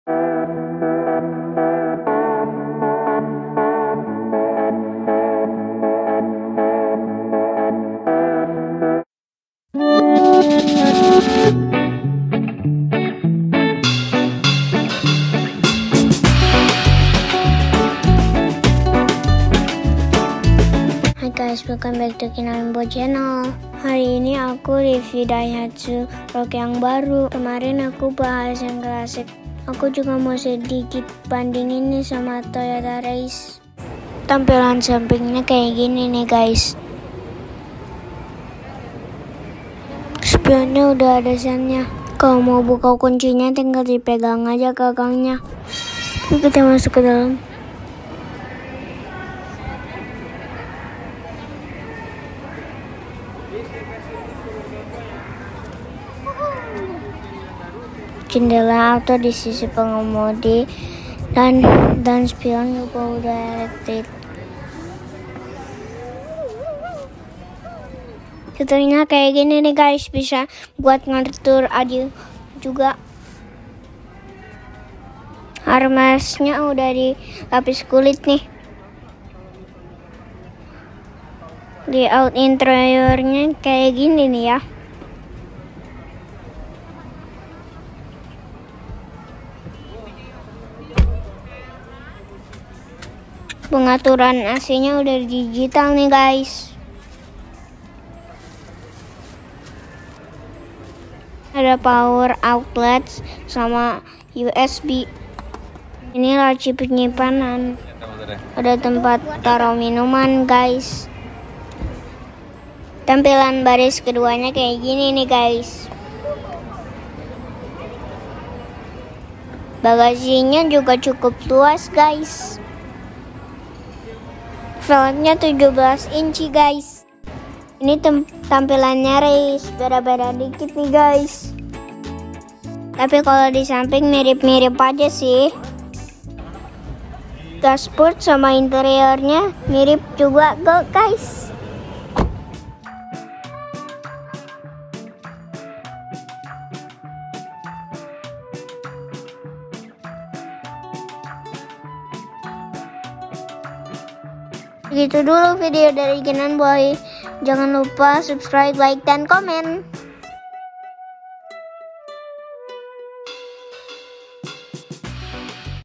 [REVIEW] TOYOTA RAIZE 2021 VS DAIHATSU ROCKY, Mending Mana ya???
Kalau sebelumnya Keenan Boi coba review Daihatsu Rocky yang classic, sekarang Keenan mau coba liat Daihatsu Rocky yang baru nih guys. Sama sedikit bandingin sama Toyota Raize kembarannya.